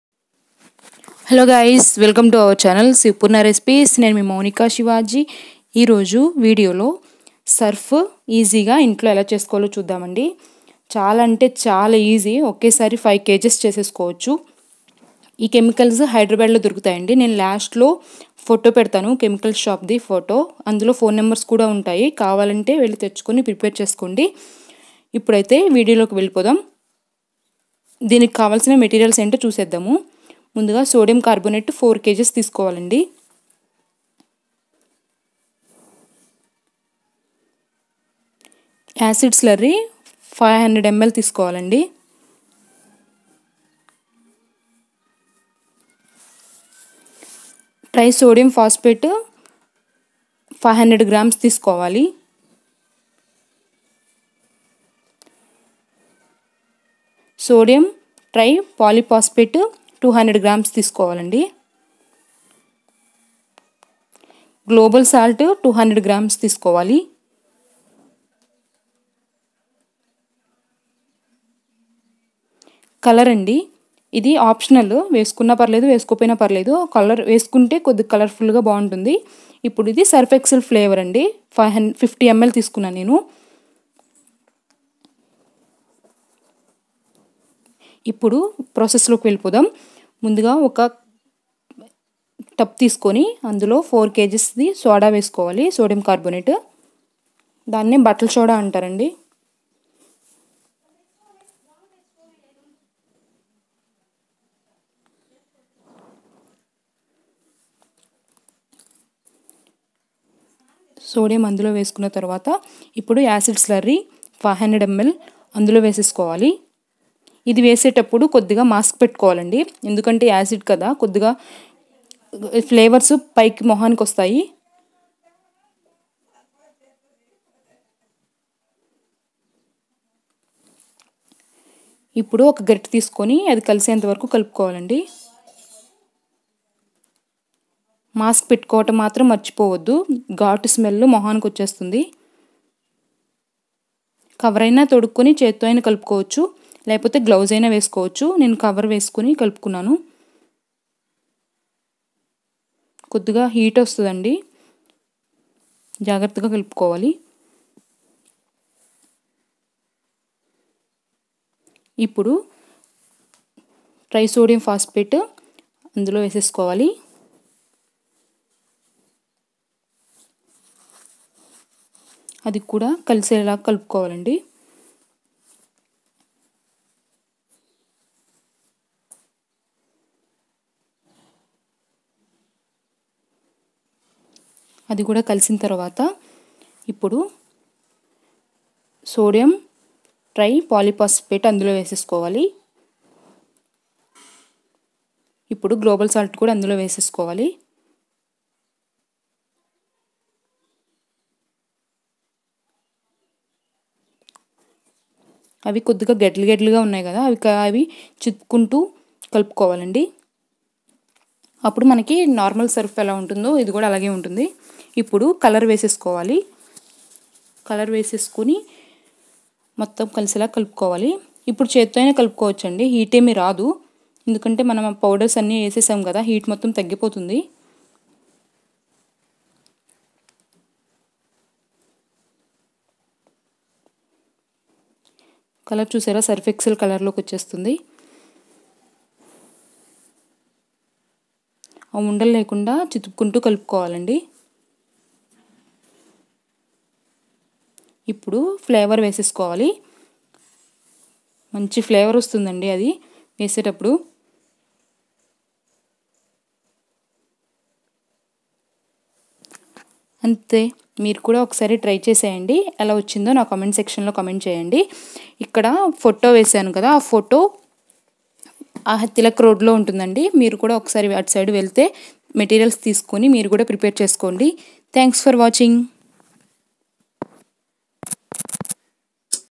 |ఇంట్లో నే సర్ఫ్ ఈజీ గా చేసుకోండి|surf making in Telugu|sivapurna recipes
surf making formula:

sodium carbonate : 4kg
acid slurry : 500ml
try sodium phosphate : 500g
sodium try poly phosphate: 200g
global salt :200g
 blue colour: 100 g
surf excel flavour : 50ml